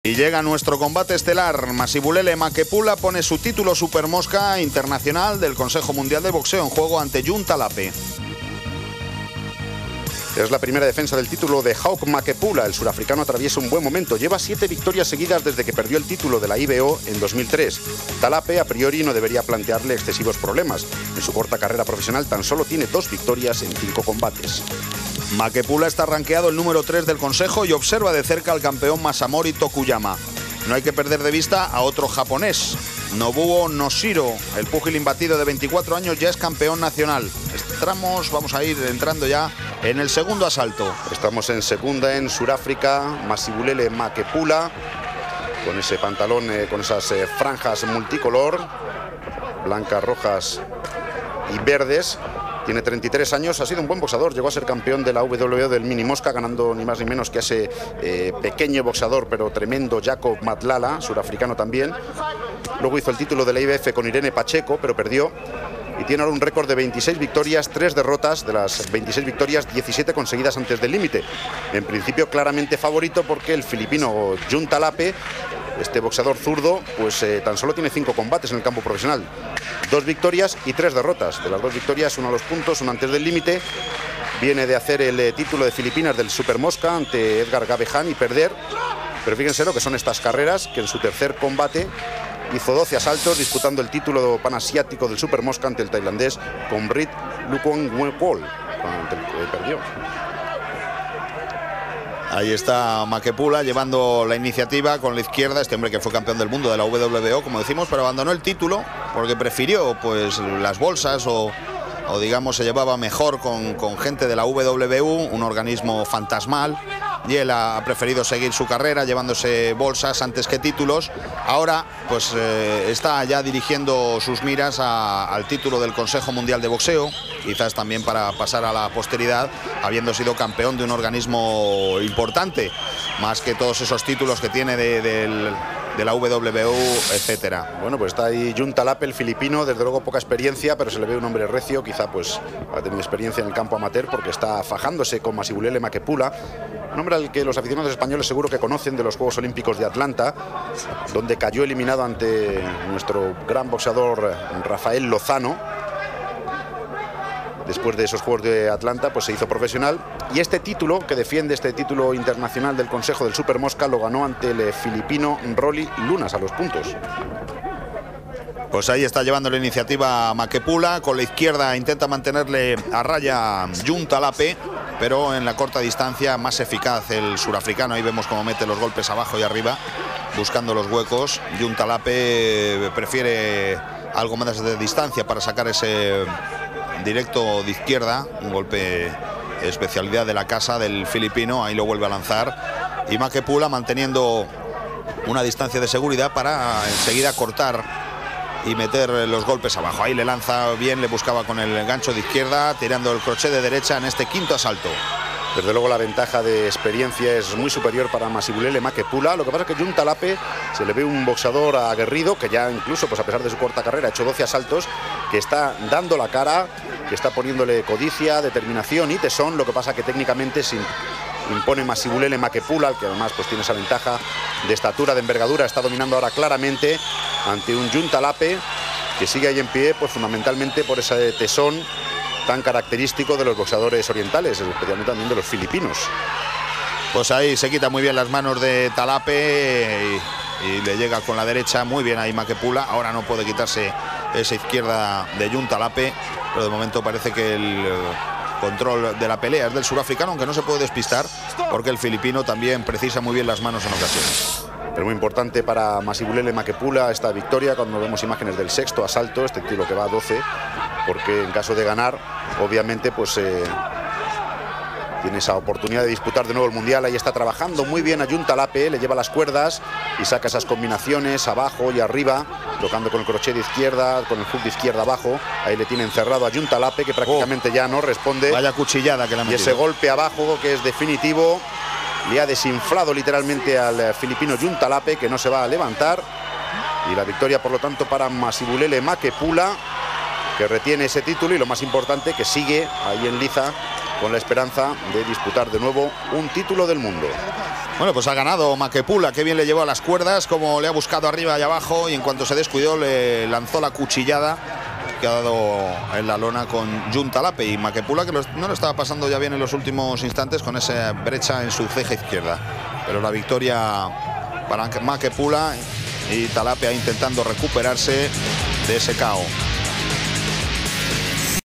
Masibulele MAKEPULA 🇿🇦 vs 🇵🇭 Jun TALAPE [10-02-2006] [HL's] [📺: Onda 6 🇪🇸]
El excampeón mundial sudafricano Masibulele Makepula se enfrenta al filipino Jun Talape en su intento por conseguir una nuevo cinturón en un peso superior.

Comentarios del desaparecido canal de televisión Onda 6 por parte de Jorge Lera y Emilio Marquiegui.